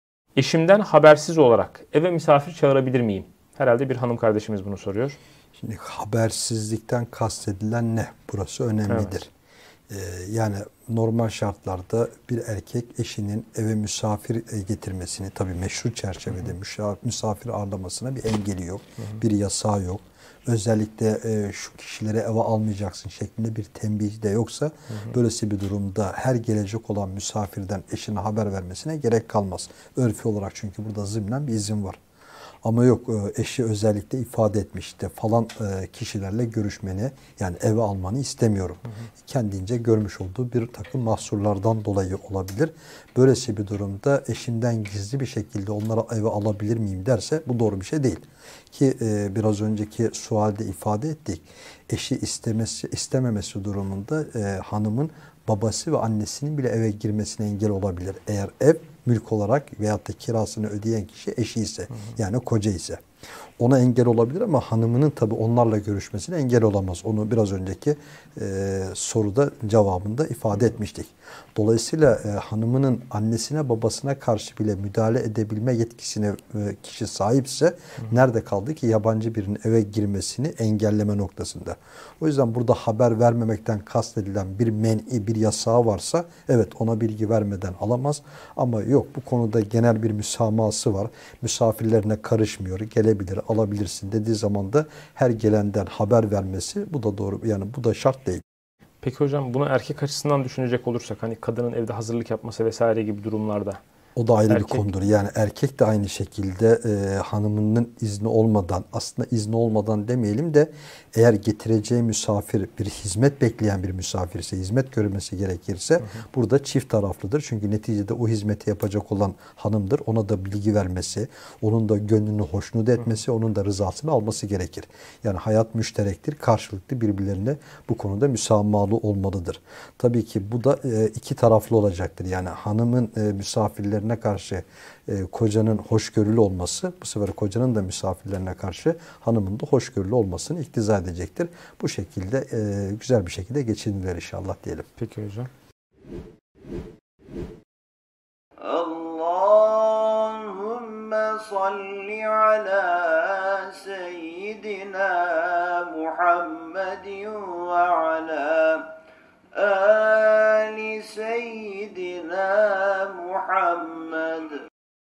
Eve Kocadan Habersiz Misafir Çağırılabilir mi? - Fatih Kalender Hoca Efendi
Eve kocadan habersiz misafir çağırılabilir mi? Kocasının şu kişileri eve alma demesine rağmen o kişilerin hanımı tarafından eve alınması caiz midir? Her gelen misafir kocaya bildirilmeli midir?

İsmailağa Fıkıh Heyeti üyesi Fatih Kalender Hoca Efendi, sizler için cevapladı.

İsmailağa NET tarafından hazırlanan video kesitlerini paylaşarak sohbetlerin daha geniş kitlelere ulaşmasına vesile olabilirsiniz.